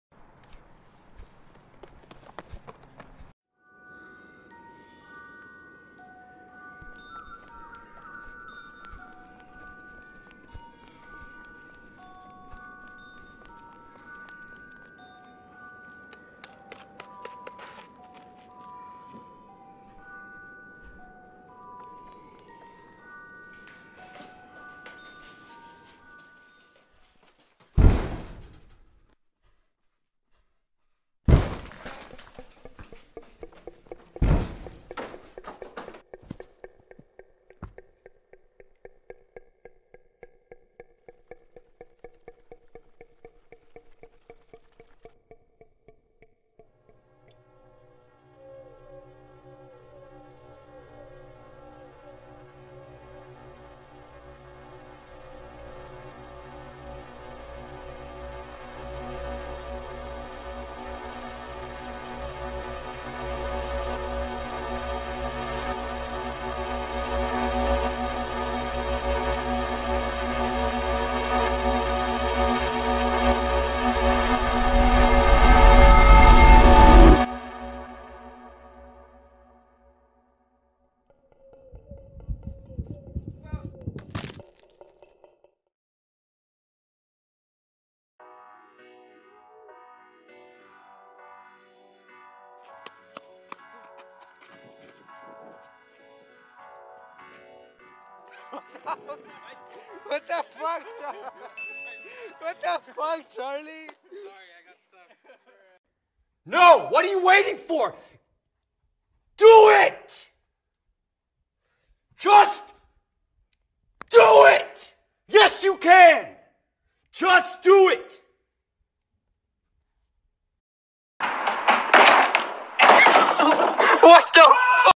Skit: Fear
Hi, We game o clock made this skit  purposely for our camera shots, we wanted to test our camera quality and such.
We decided that it would be cool to release this to see what yall thought about it..
We are wanting to do a bigger project soon, and then grow from there but for now
-Film O Clock